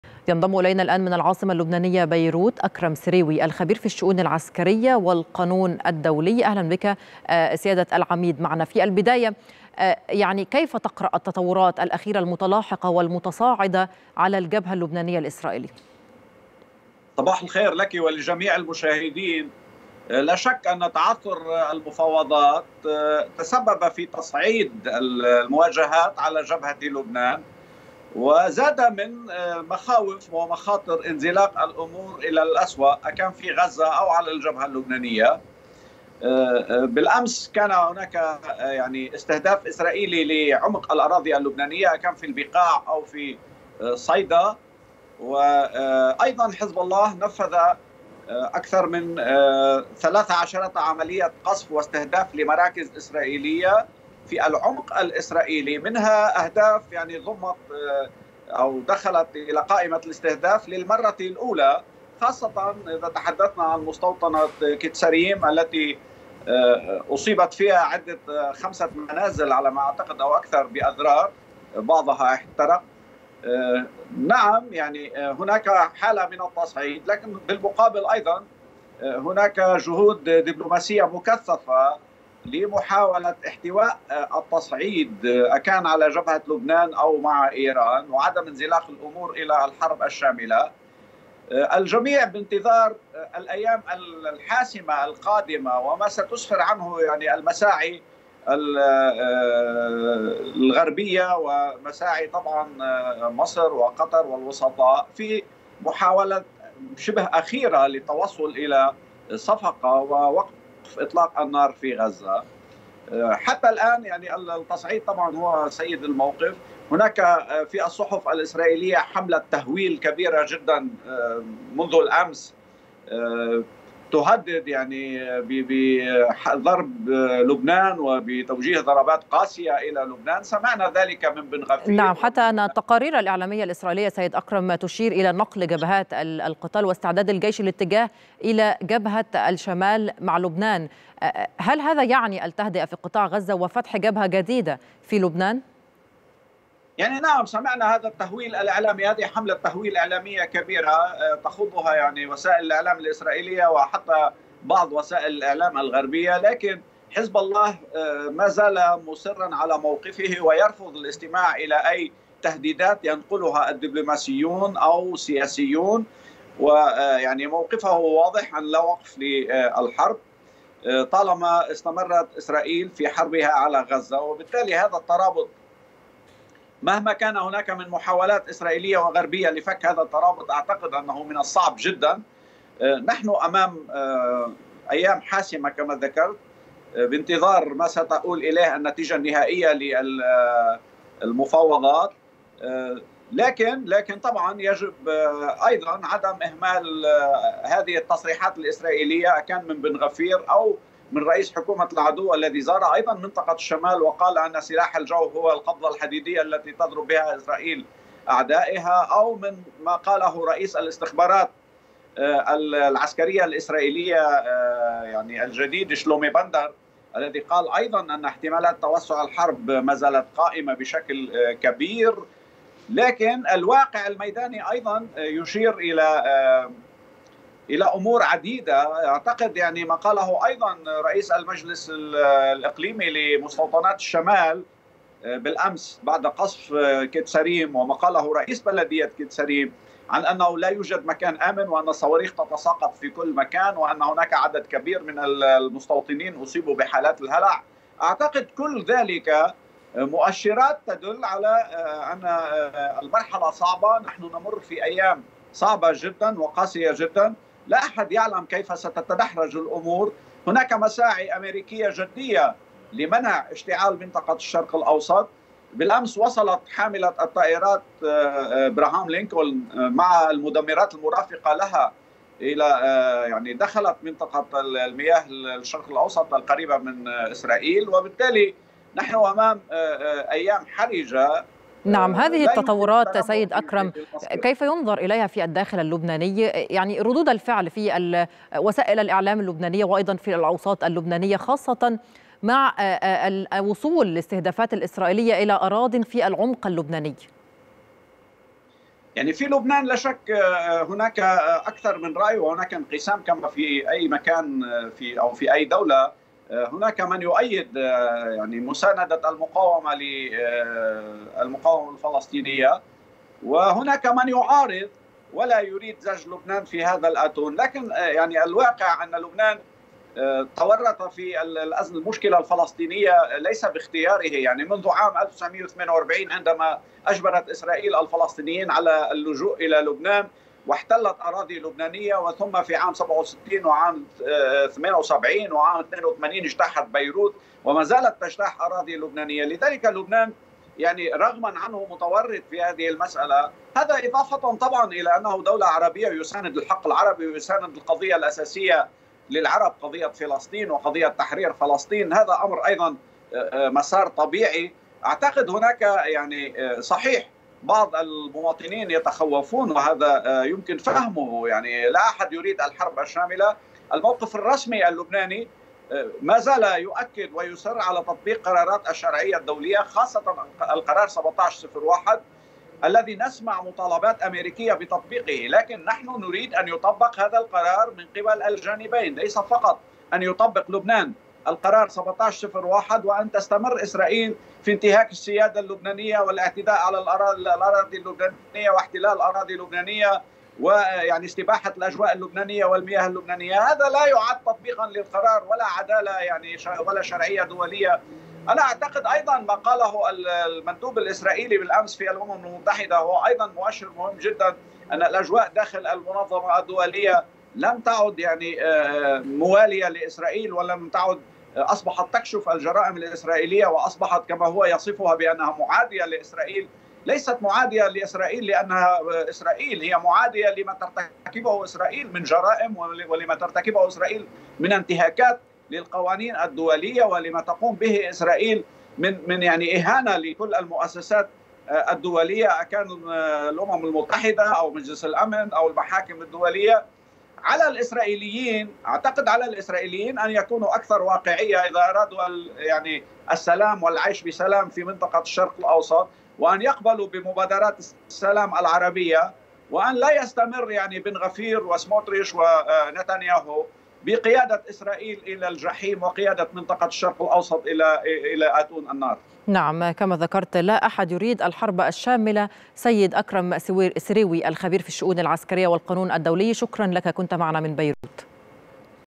خبير عسكري لبناني: تعثر المفاوضات تسبب في تصعيد المواجهات على الجبهة في لبنان بين حزب الله وإسرائيل
أكرم سيروي الخبير العسكري اللبناني: تعثر المفاوضات تسبب في تصعيد المواجهات على الجبهة في لبنان بين حزب الله وإسرائيل

22/8/2024